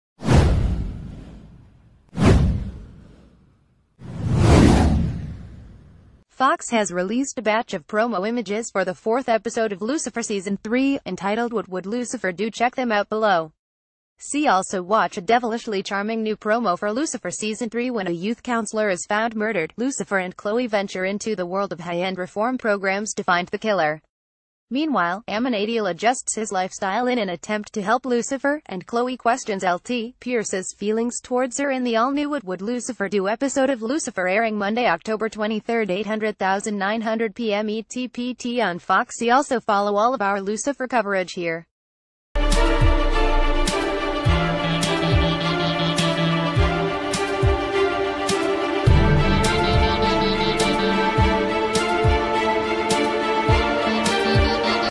Breaking News | Promo images for lucifer season 3 episode 4 – ‘what would lucifer do?’
Promo images for lucifer season 3 episode 4 – ‘what would lucifer do?’

Fox has released a batch of promo images for the fourth episode of Lucifer season 3, entitled ‘What ...